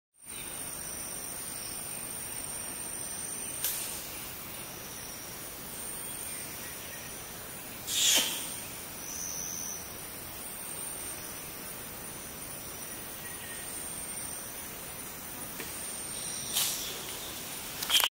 Adult female Bornean orangutan kiss-squeaks on leaves
Adult female orangutan kiss-squeaks on leaves
Mungku Baru Education Forest, Central Kalimantan, Indonesia